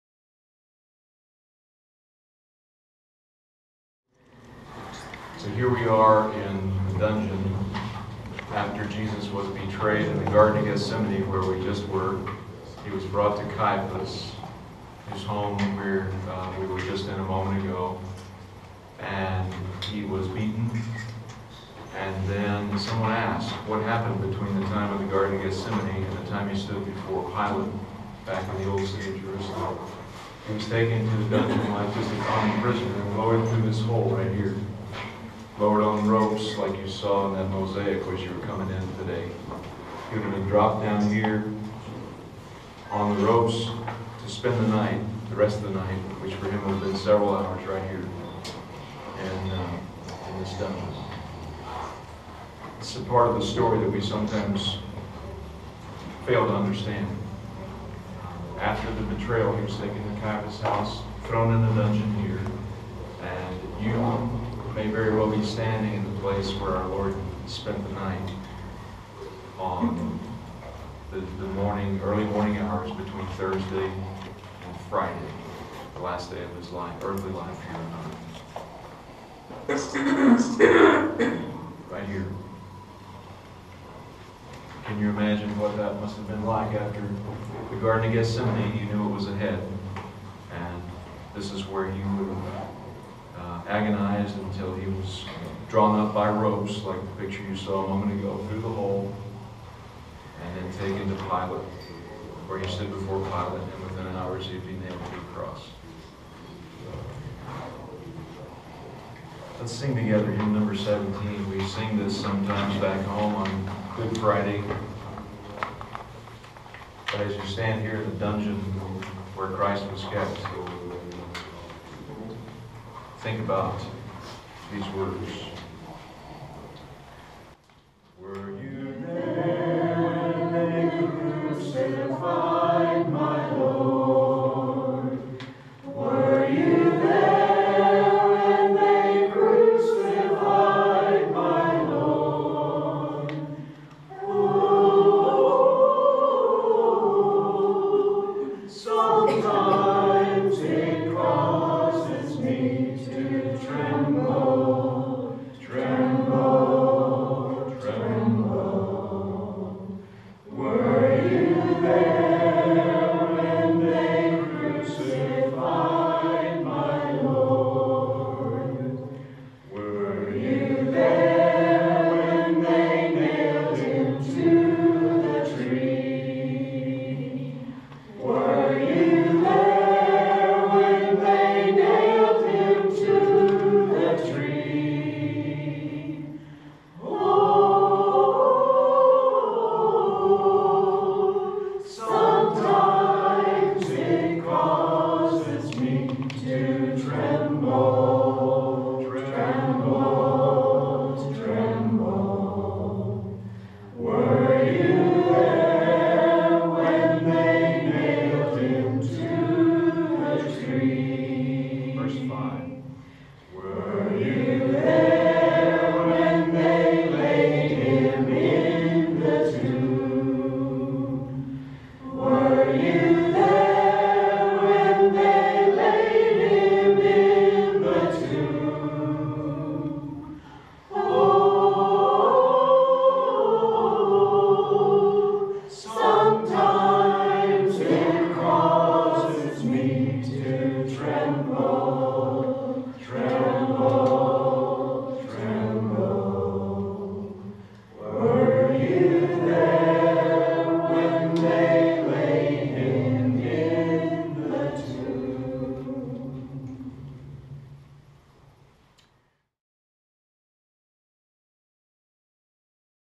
From Israel to Claremore: Part 14 - In the High Priest's House
Pastor Ray shares with us from the dungeon in the House of Caiaphas, underneath the Church of St. Peter in Gallicantu in Jerusalem, Israel.